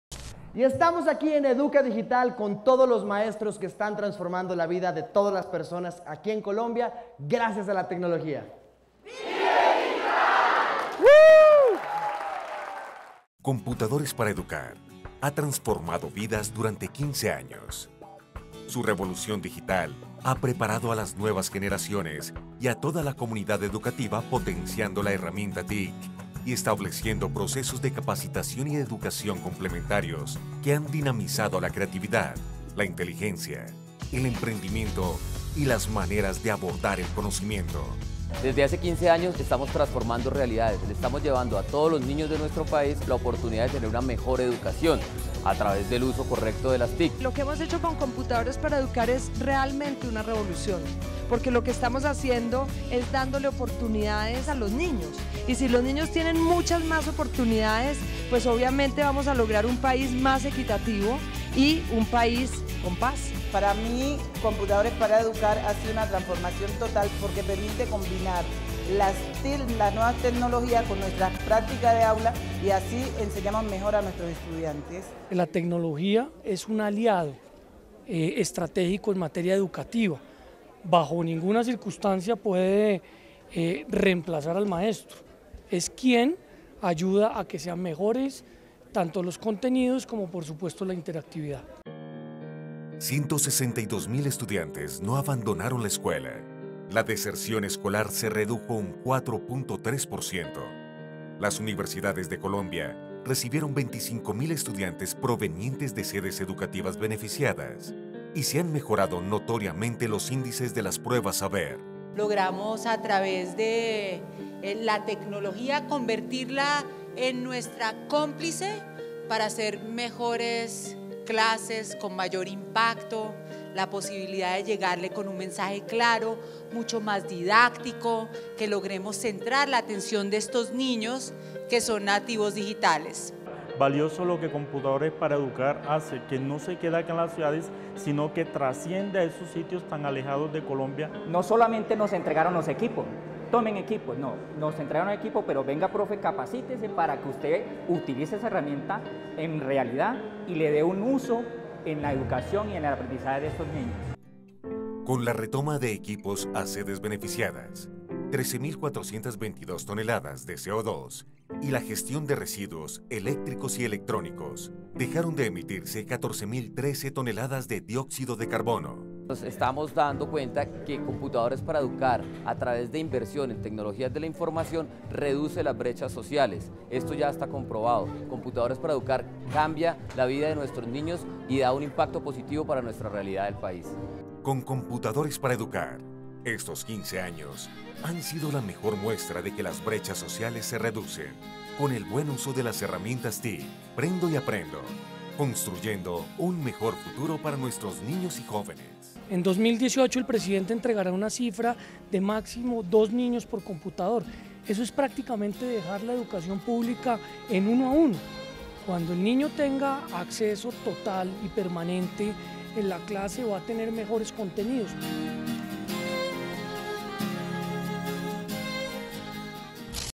Maestros transforman vidas con EducaDigital C20 - N1
Con herramientas TIC, los docentes potencializan la creatividad y el aprendizaje de sus alumnos.